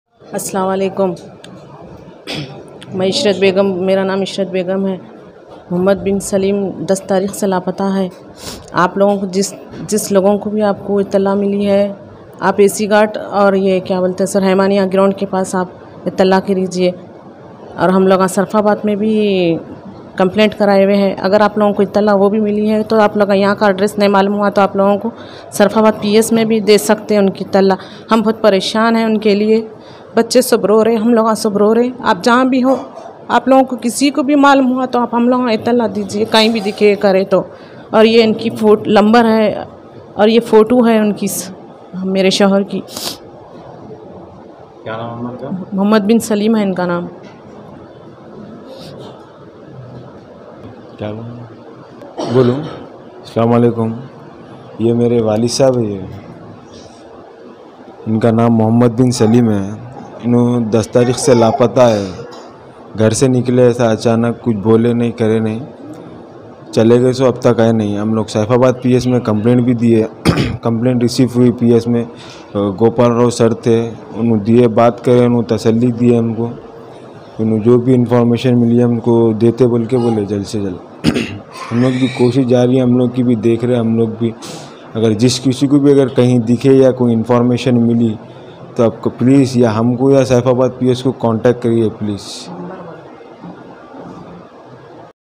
Saifabad Police station limit AC guard ma ek shakhs 10 tarikh din ke 2:00 baje se lapata hai jinka
Saifabad Police station limit AC guard ma ek shakhs 10 tarikh din ke 2:00 baje se lapata hai jinka Naam Mohammed bin Salim bata ya ga jinki age 50 years hai agar kisi ko bhi najar aaye To sadabad police station iski tala de